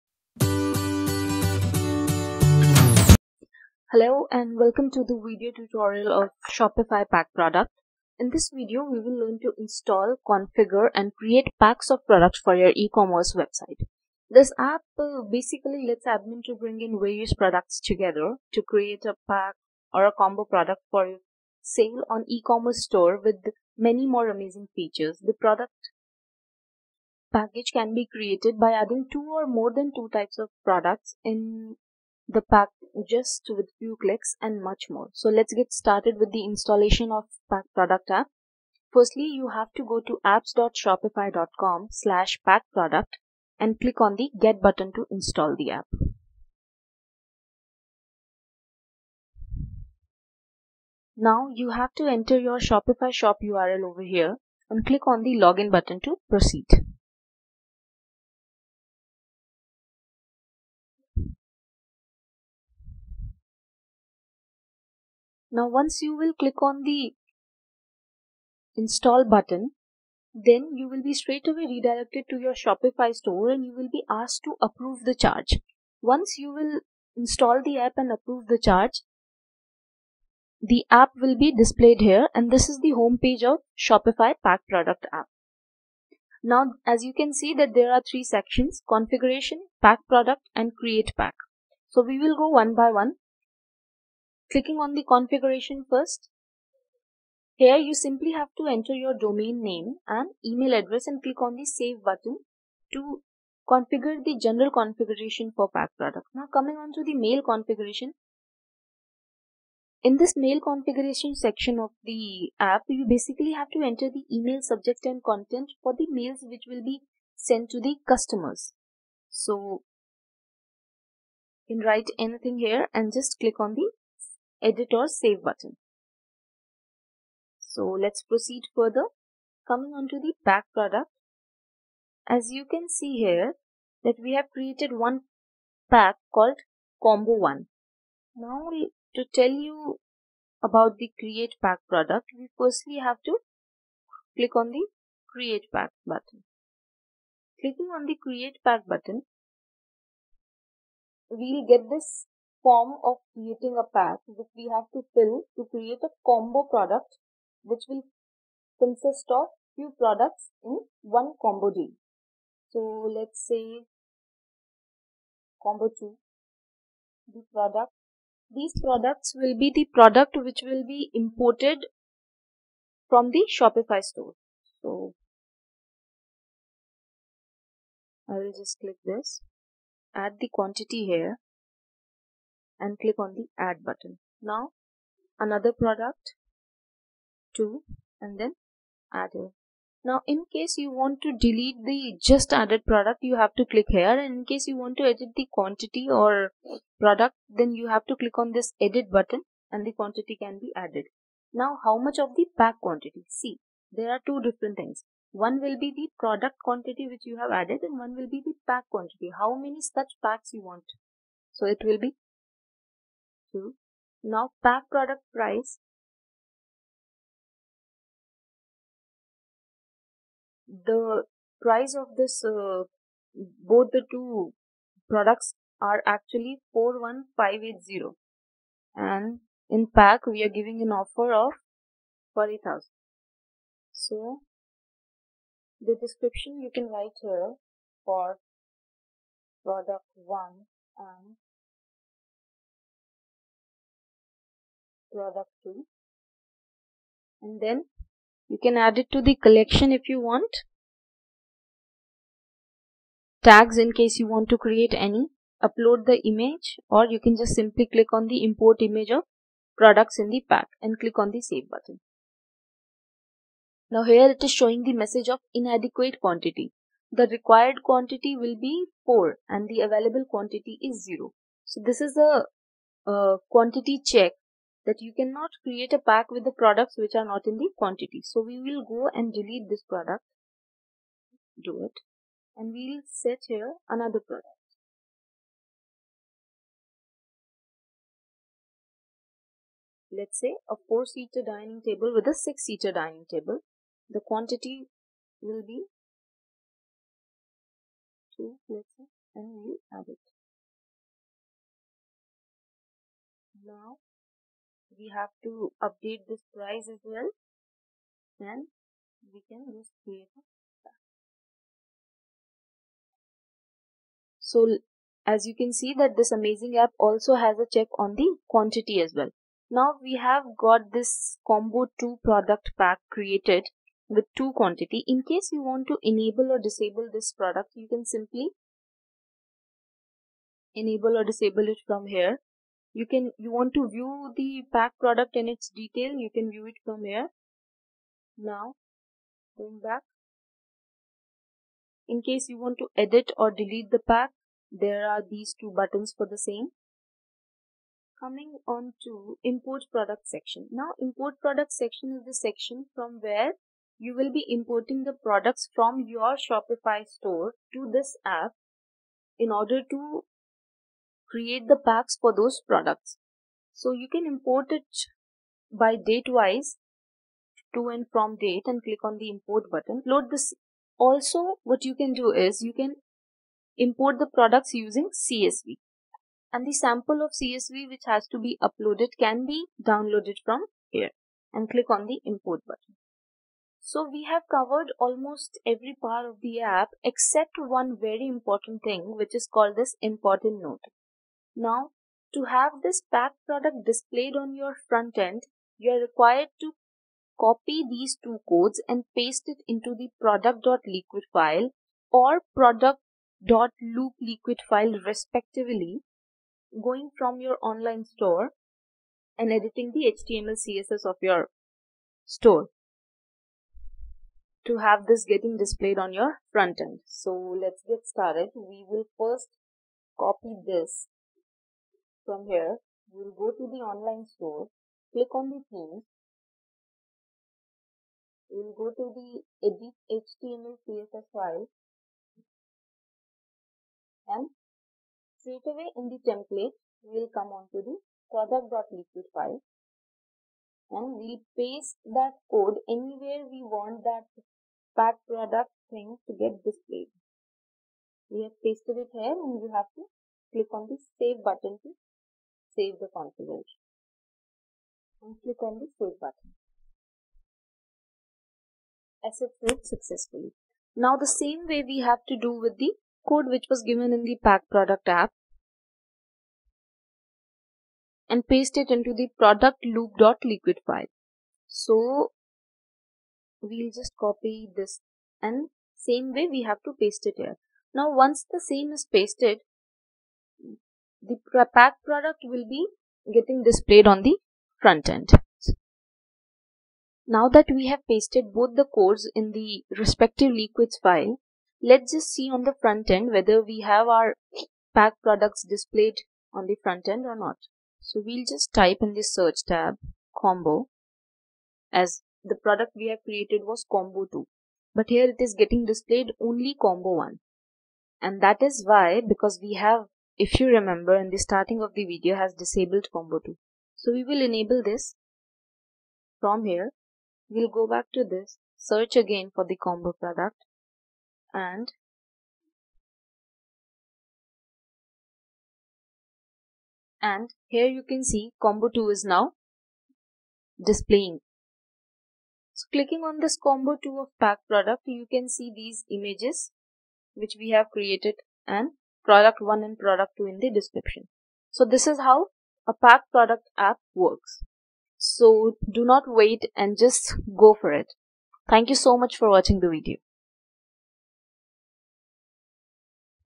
Pack Product App for Shopify: Installation and Configuration
In this video, Webkul demonstrates the installation and configuration of Pack Product App for Shopify.
This app lets store owner bring in various products together to create a pack/combo product for sale on eCommerce stores with many more amazing features.

2. Raise a Ticket via our HelpDesk system